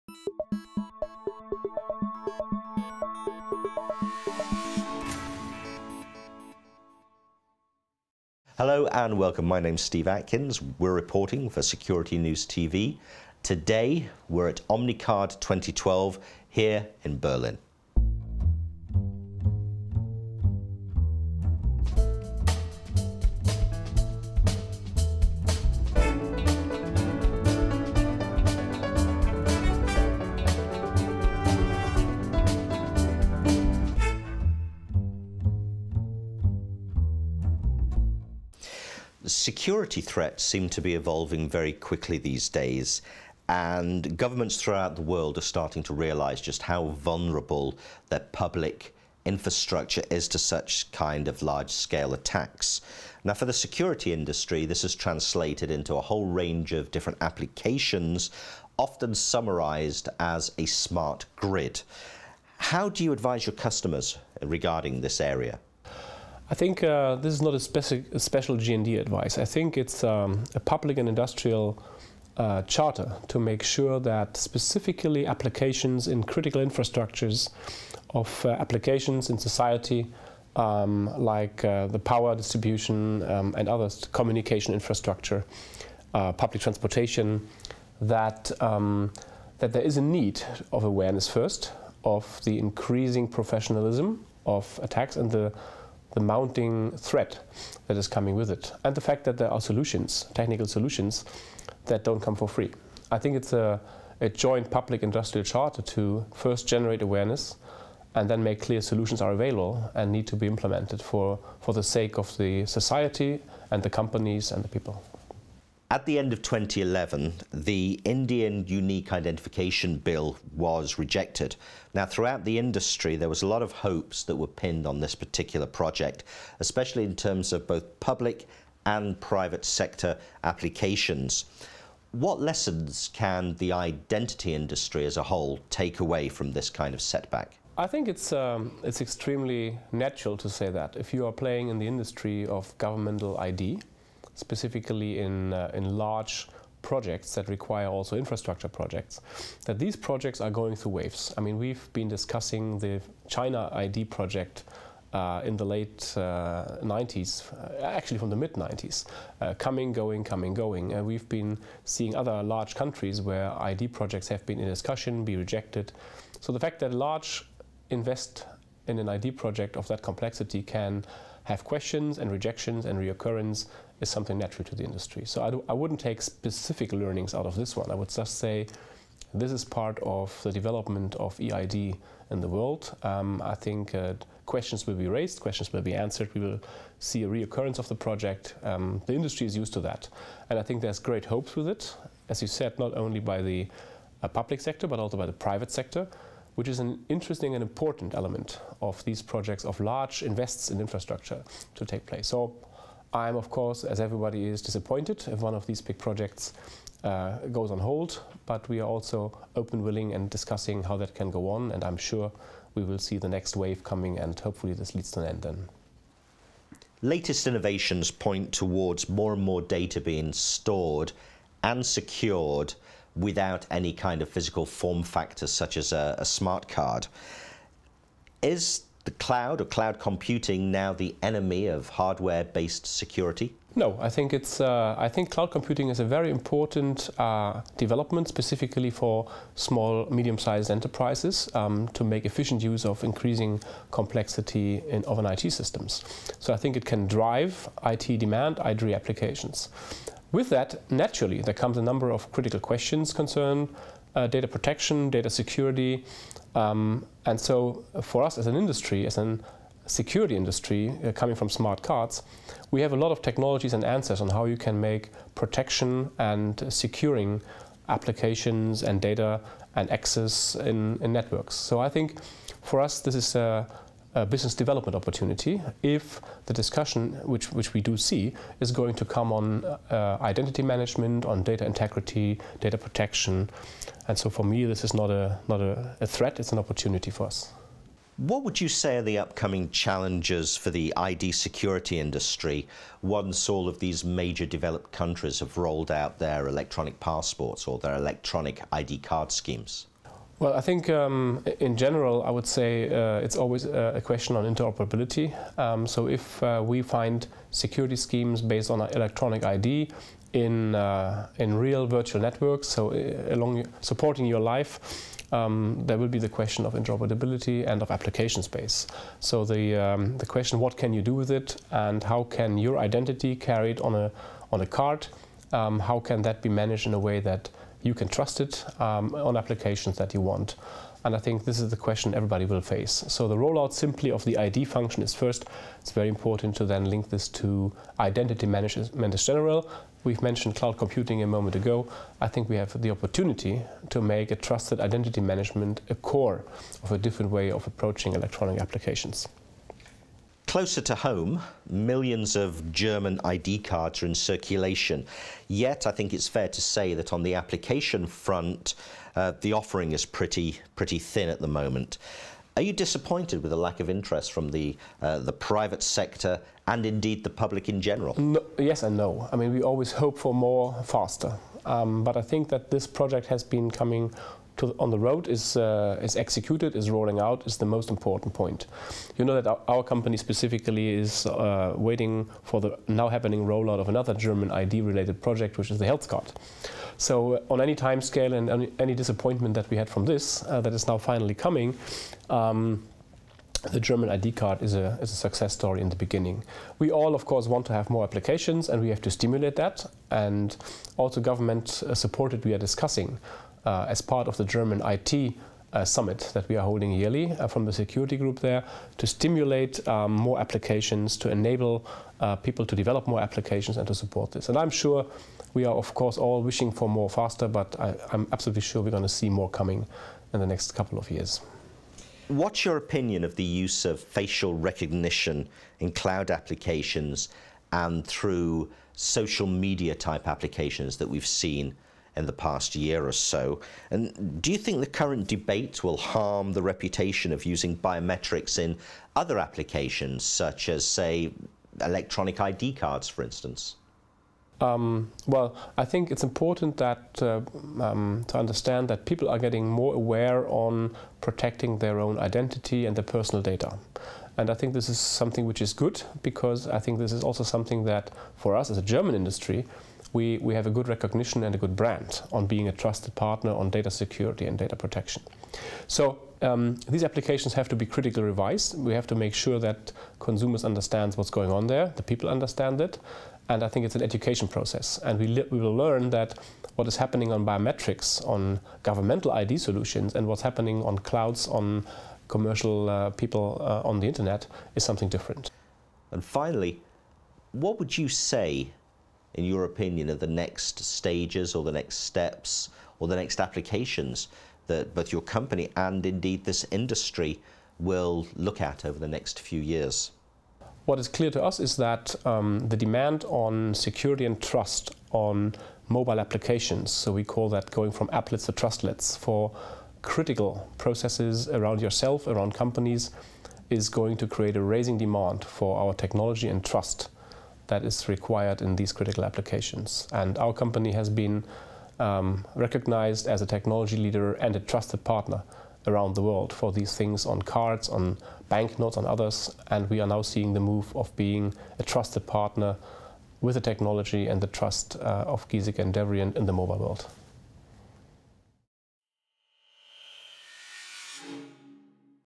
Giesecke & Devrient: Moving from applets to trustlets
Security-News TV met up with Dr. Karsten Ottenberg to question him on some of the challenges that lay ahead for Giesecke & Devrient: Smart Grid, Cloud Computing and the chicken-and-egg scenario behind the lack of applications for the German ID card. Ottenberg, who has been CEO of the family-owned company since 2005, has been one of the founding fathers of NFC technology during his time at Philips Semiconductors (now NXP) and now faces the massive task of developing solutions that help secure an increasingly mobile world.